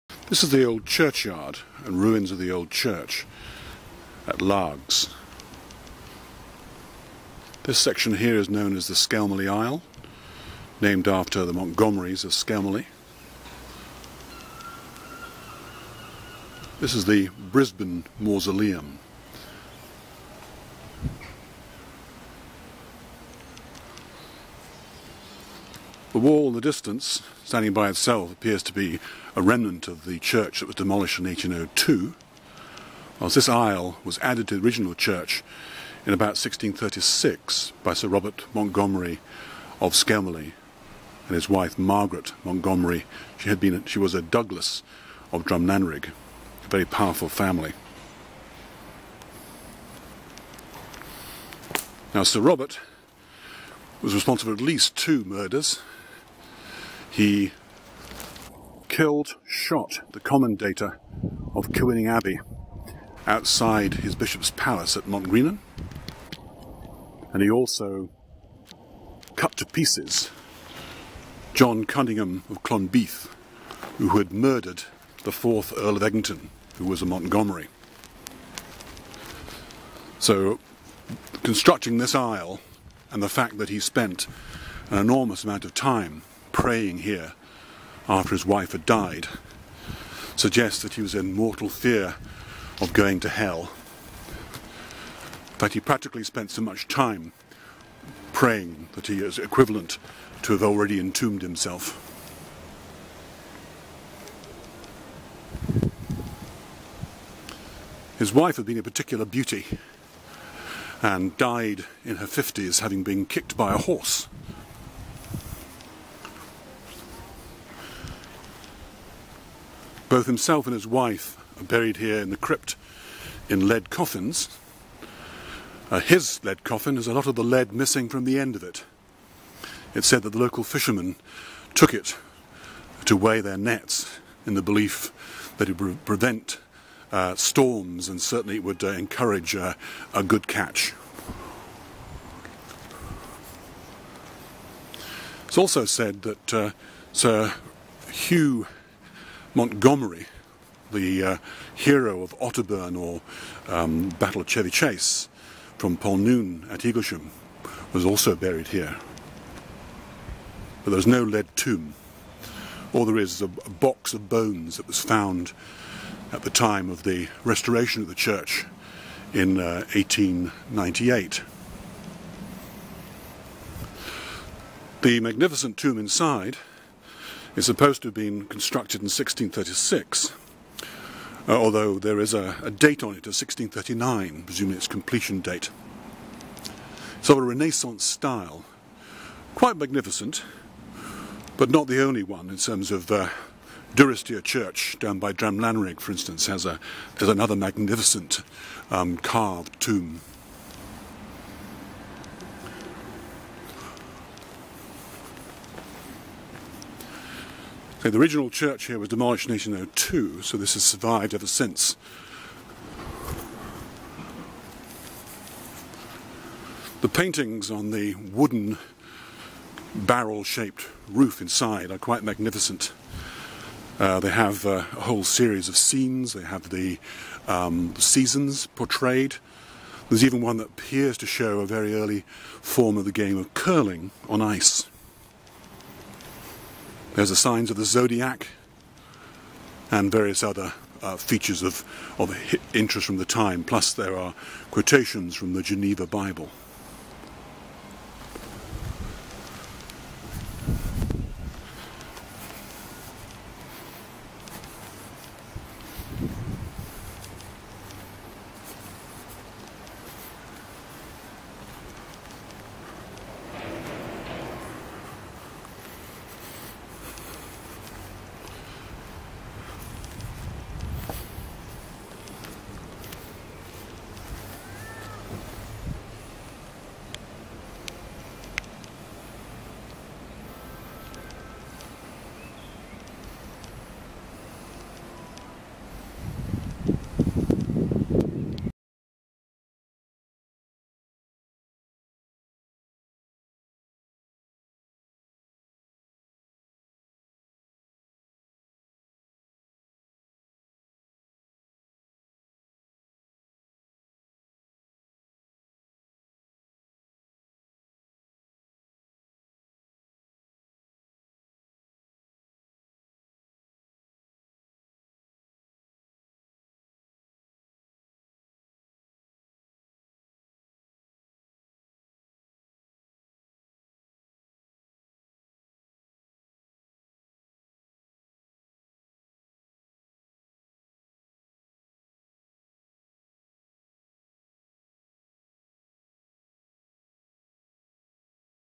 The Skelmorlie Aisle - History, Painted Ceiling Panels and the Monument
A spectacular example of 17th century Renaissance style stone carving and ceiling painting created for Robert and Margaret Montgomerie of Skelmorlie Castle. The old kirk of Largs was demolished circa 1802 and the aisle preserved. The aisle has the 19th century Brisbane Mausoleum located nearby and the whole is under the care of Historic Scotland assisted by the Largs & District  History Society.